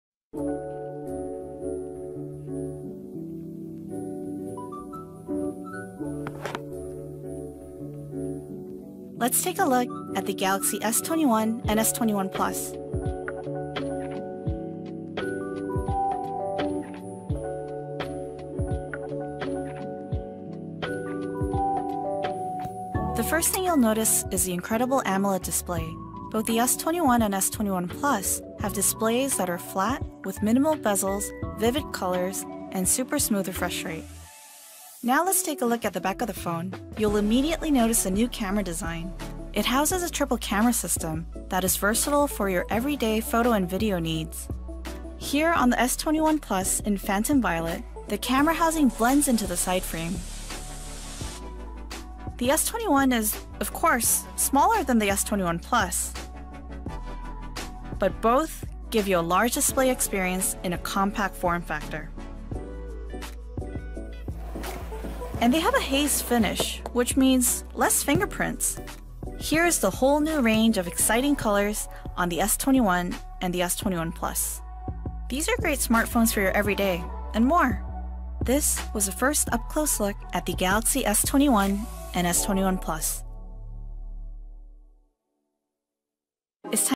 Samsung Galaxy S21 Hands On | Samsung S21 Plus | First Impression
Samsung Galaxy S21 & Samsung Galaxy S21+ first impression Hands-on video. The phone has tripe rare cameras and front-facing camera is 10MP.